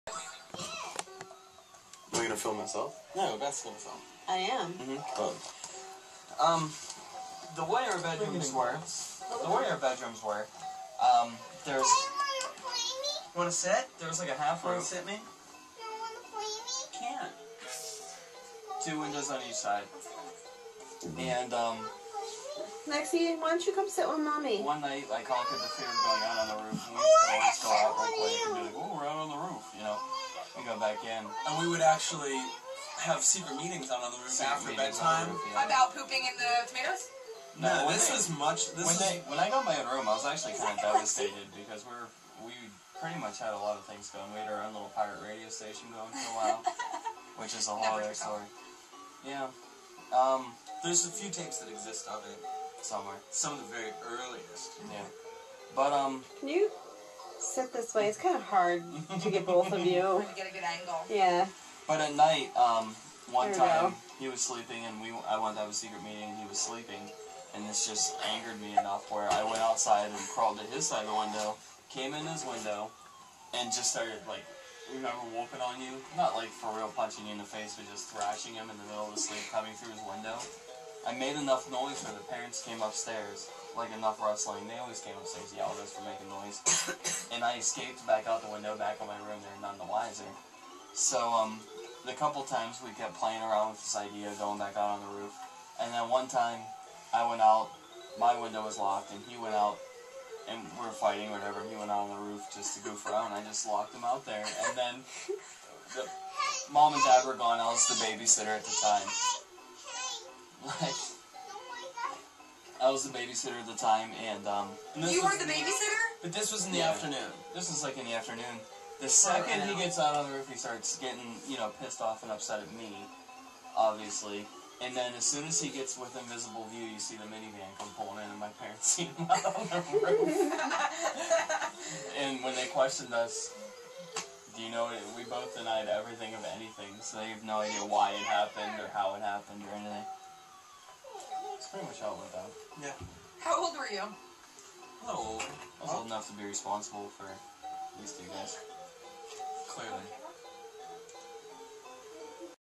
Nostalgia Locked Out on Roof
The Clemente brothers explain the architecture of thier childhood home